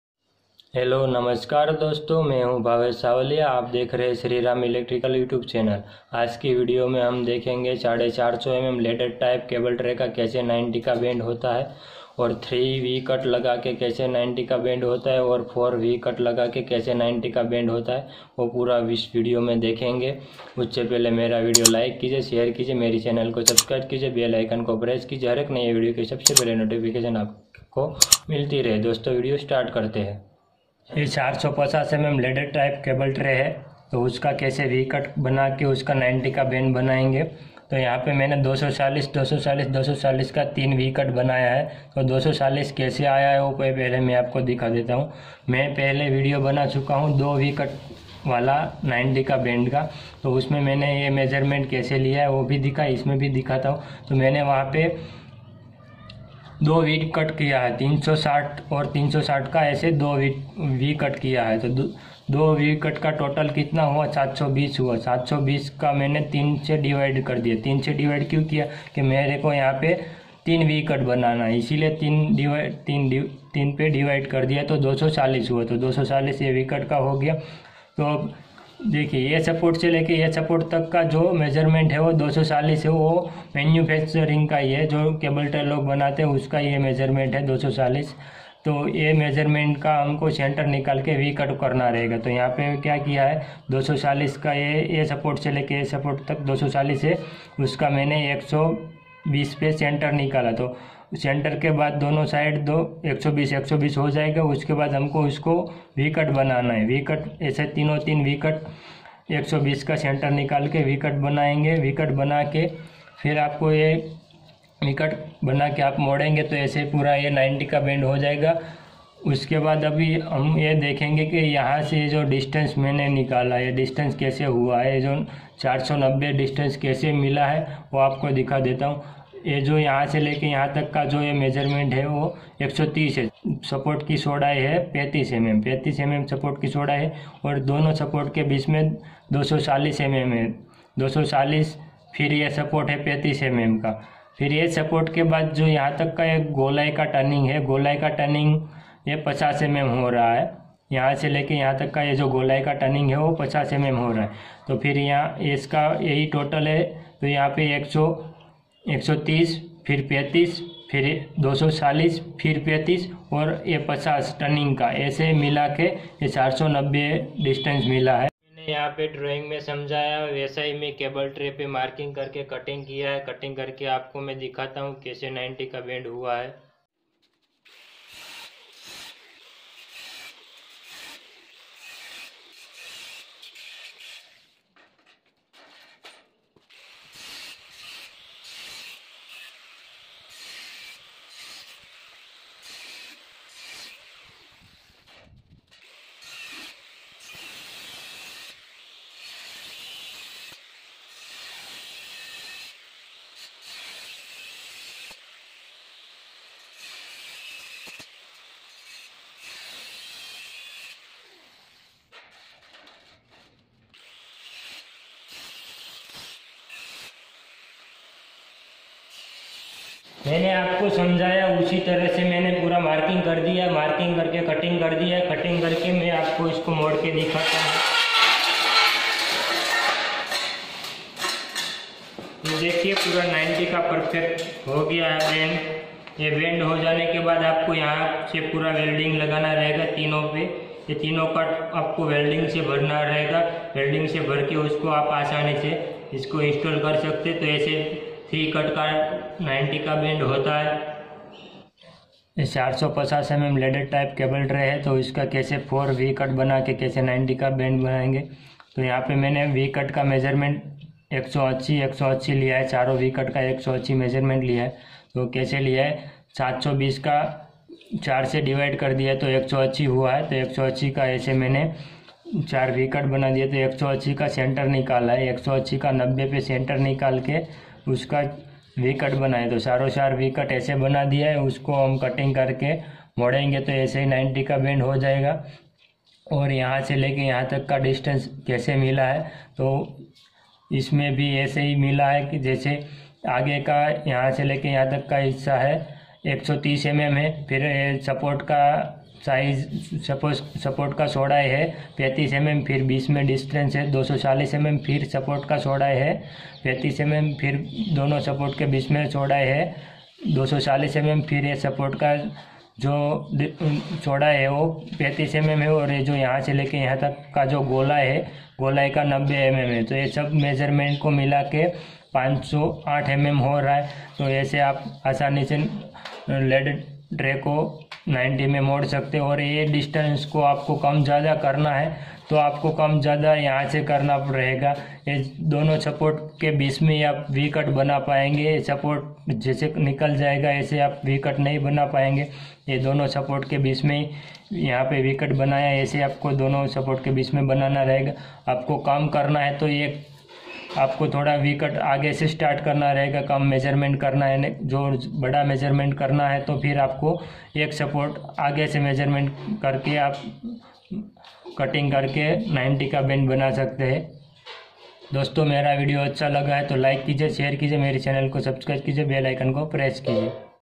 Ladder Type 450mm Cable Tray 3 Cuts and 4 Cuts 90 Degree Bend | लेडर टाईप केबल ट्रे 3कट,4कट 90°बेंड
Hello Viewers  May Name is Bhavesh Savaliya And Welcome to May YouTube Channel

Dosto Aaj ki Is Video Me Maine Aapko Ye Bataya Hai Ki  Ladder Type Cable Tray ko 90 Degree me keise mode or 3 Cuts And 4 Cuts lagakar 90 Degree me keise moda Jada Jata hai  Droing ke jariye Dikhaya hu Or Practical Cable Tray ko katting karke 90 Degree ka Bend Dikhaya hai I Hope Aapko Ye Video Bahut Pasand Ayegi Thank You So Much.

May social media handles. Make sure you follow me everywhere